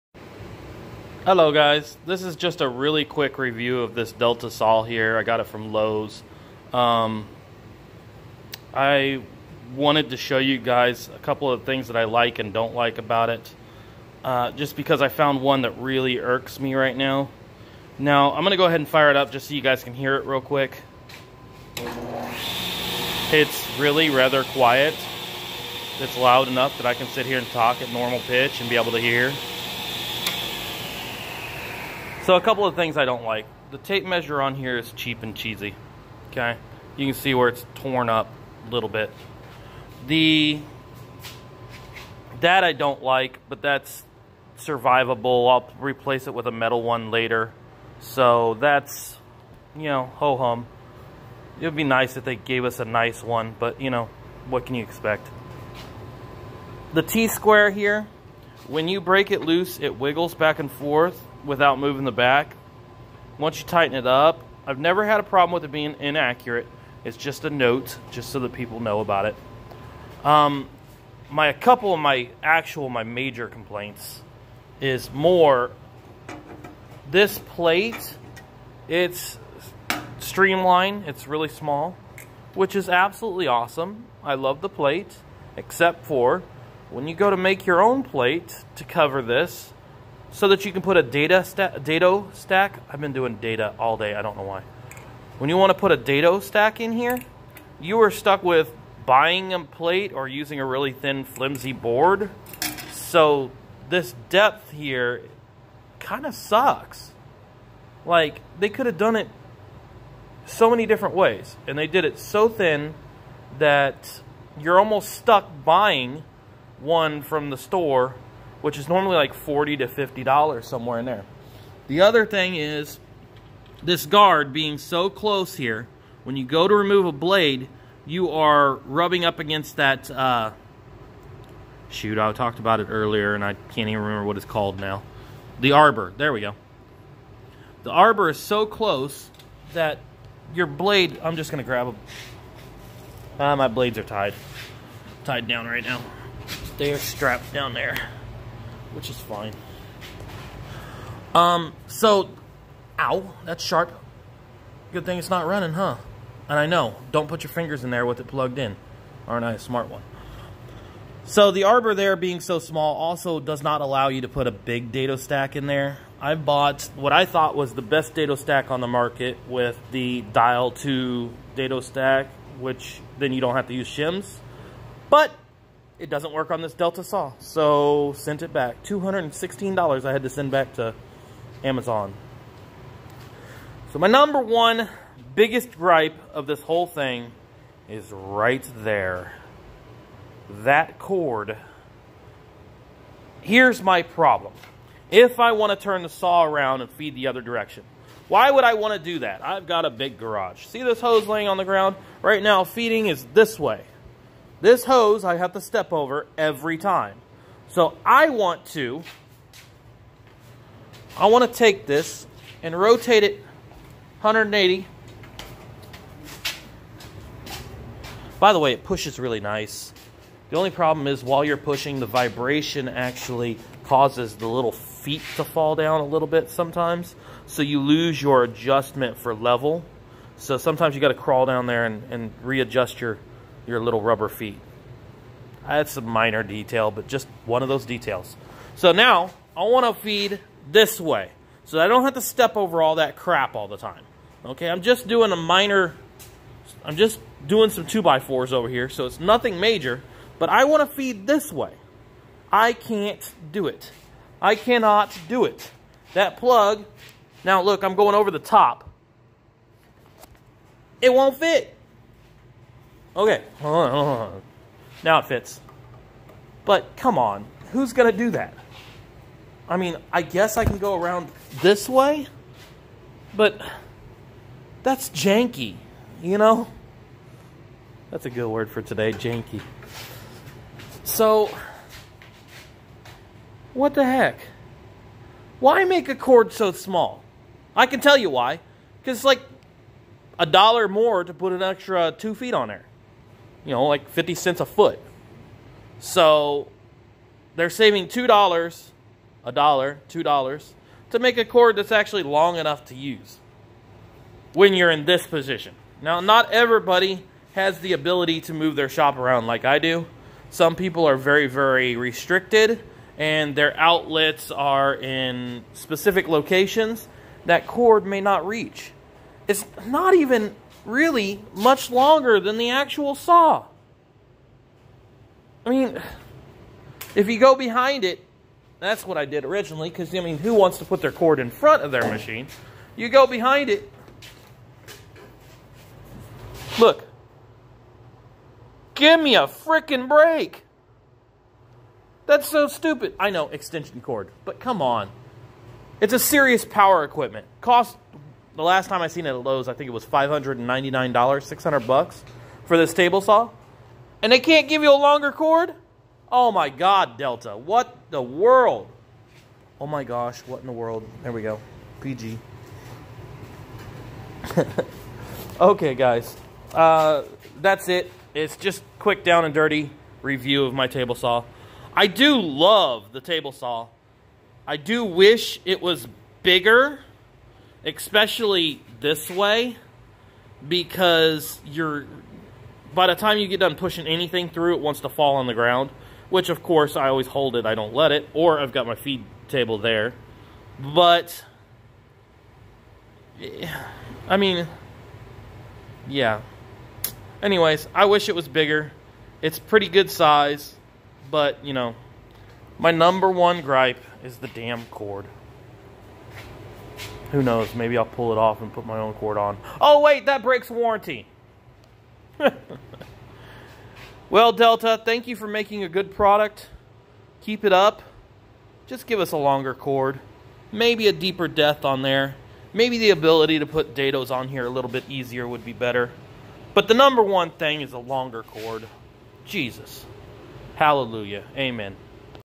delta 6 month review
I am reviewing my Delta saw that I got a close. Please ignore the blemishes on top of my soul I left it too close to an open window during the rain.